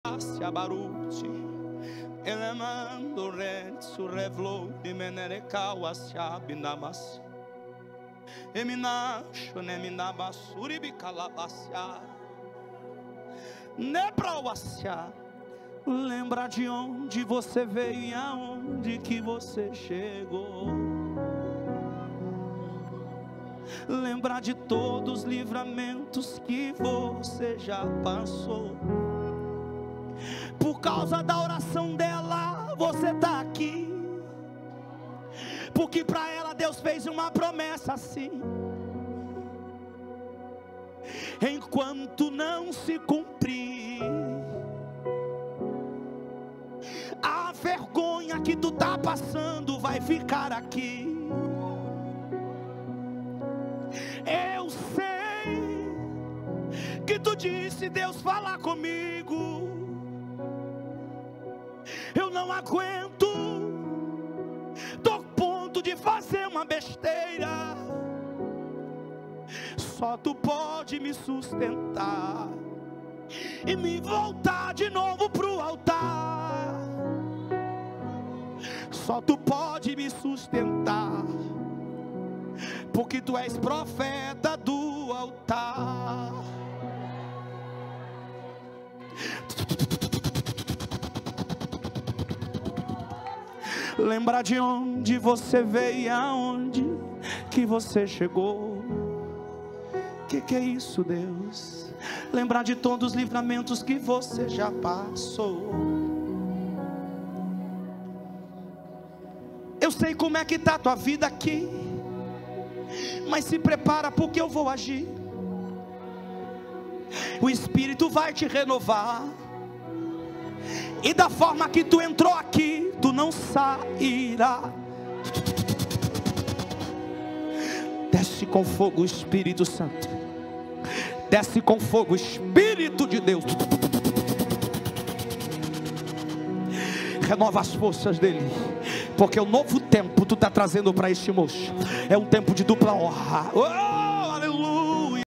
PROFETA REVELA CANTANDO PRA ESSE MOÇO! - Pr David Lacerda
❌ Uma das revelações mais fortes que você verá hoje!

Pastor David Lacerda revela algo poderoso neste vídeo! Veja, curta e compartilhe!